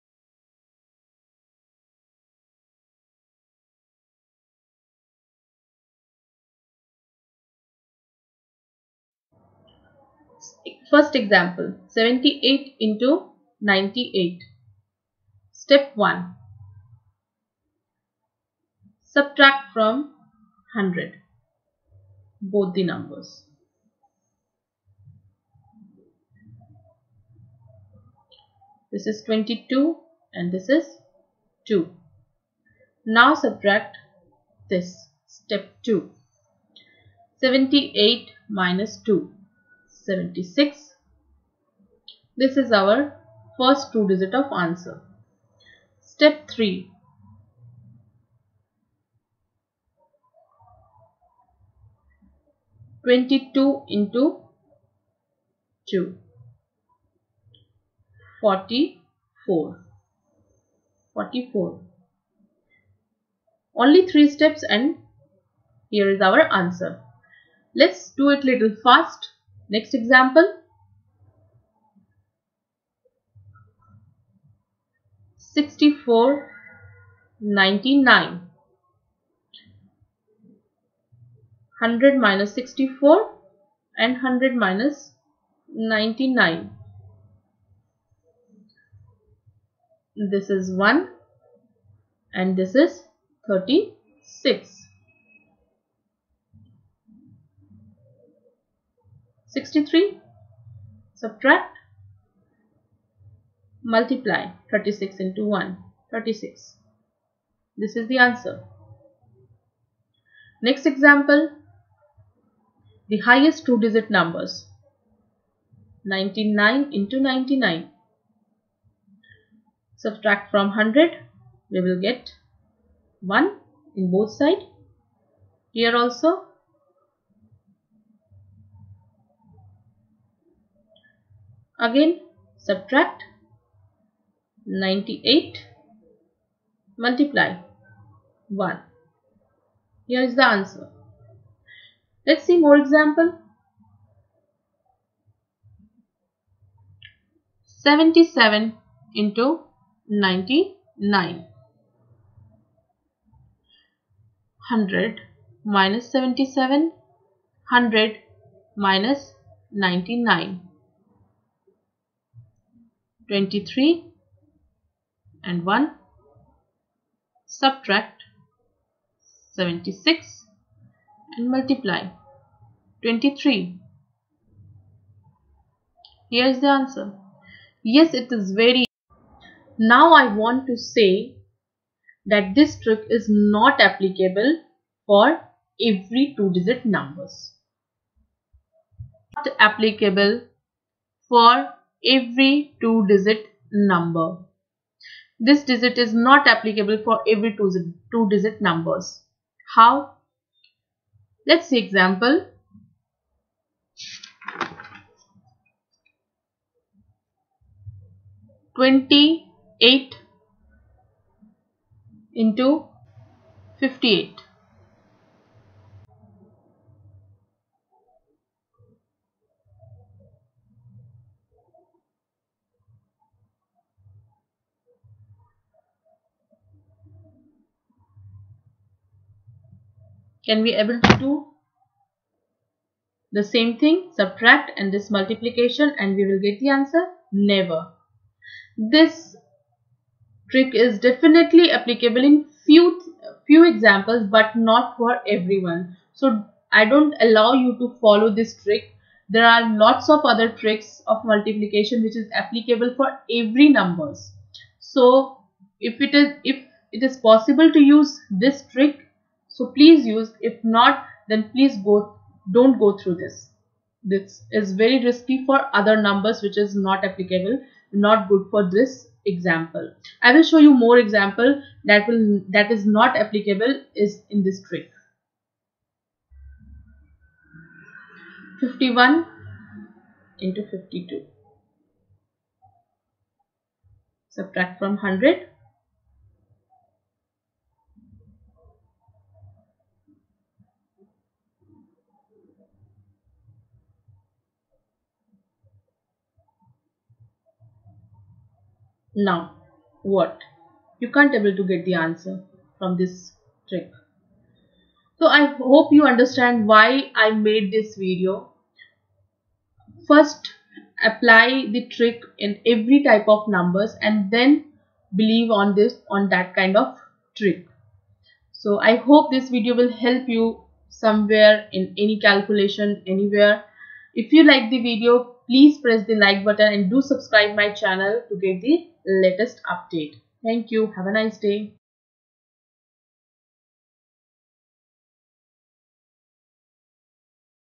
Calculation trick not applicable everywhere
Every calculation trick not working for every number.
Here is an example of a trick which is not applicable everywhere.